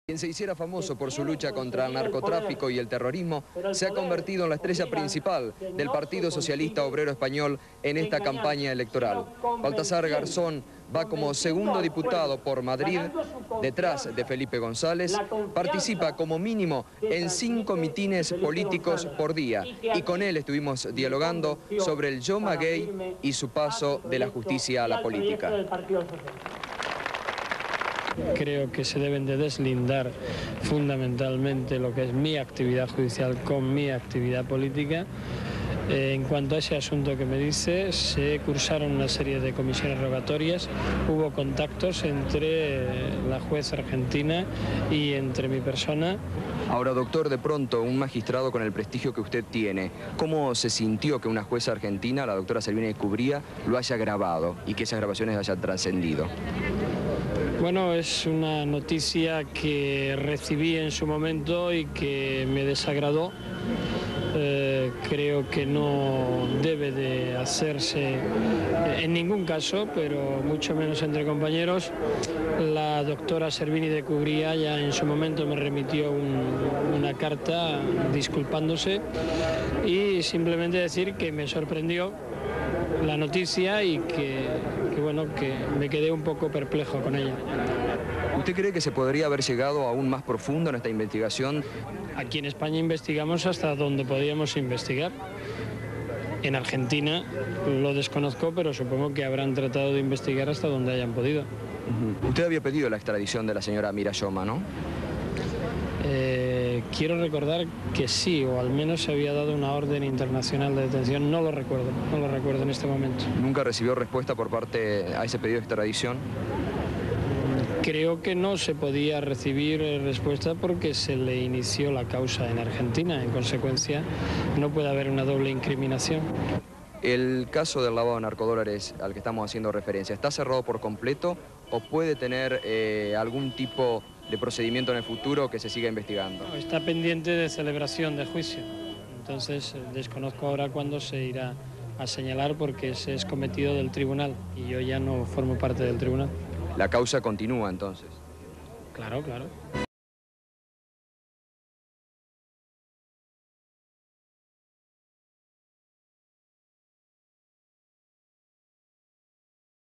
Reportaje al ex Juez Baltasar Garzon Funes - Caso Yomagate - DiFilm (1993)
CONSULTAS PARA ADQUIRIR VIDEOS A: - Reportaje a Baltasar Garzon Funes (ex juez) sobre el caso Yoma, narco dolares, Servinide cubria y su juicio. (Gustavo sylvestre)
Fecha: 1993
Duración:
Código: V-01664